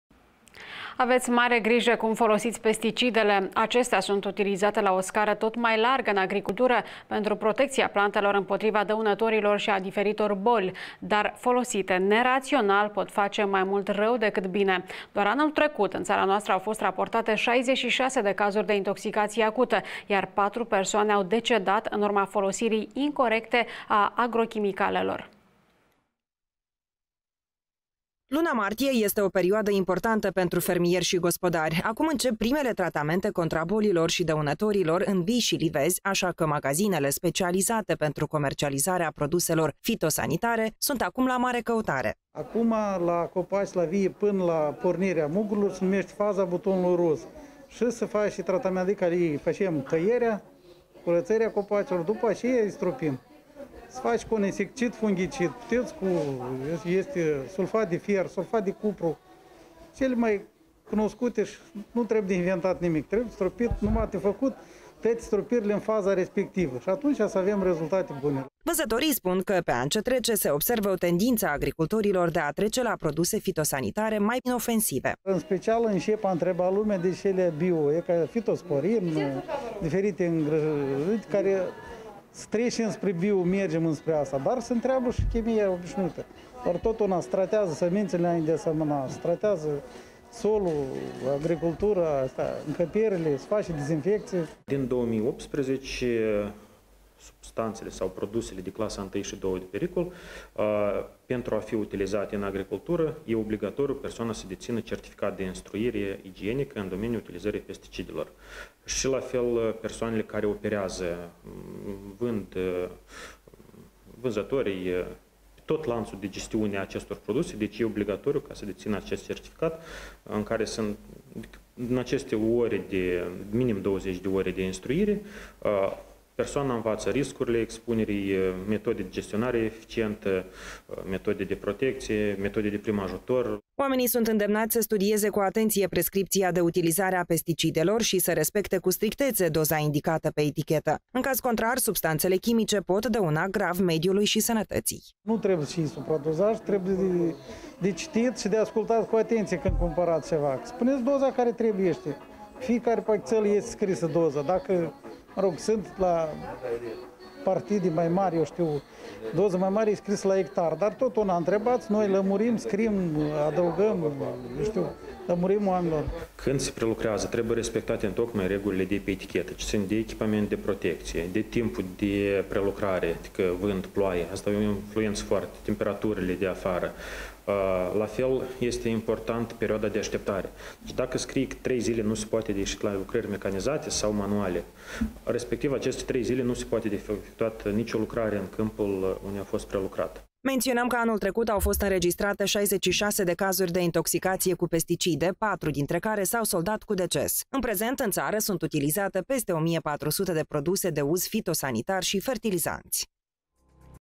Pesticidele, între utilitate și pericol
Aveți mare grijă cum folosiți pesticidele. Acestea sunt folosite la o scară tot mai largă în agricultură pentru protecția plantelor împotriva dăunătorilor și a diferitor boli. Dar utilizate nerațional, pot face mai mult rău decât bine. Doar anul trecut în țara noastră, au fost raportate 66 de cazuri de intoxicație acută, iar 4 persoane au decedat, în urma folosirii incorecte a agrochimicalelor.